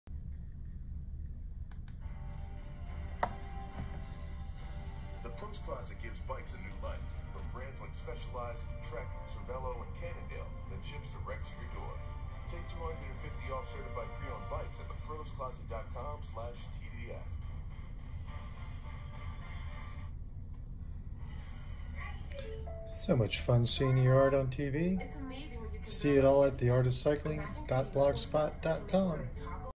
Just a fun short of my Tour de France cycling art on TV
So for four years now NBC Sports has been sharing my TourdeFrance cycling art during their broadcast of the race. They have shared it on every stage for the last three years!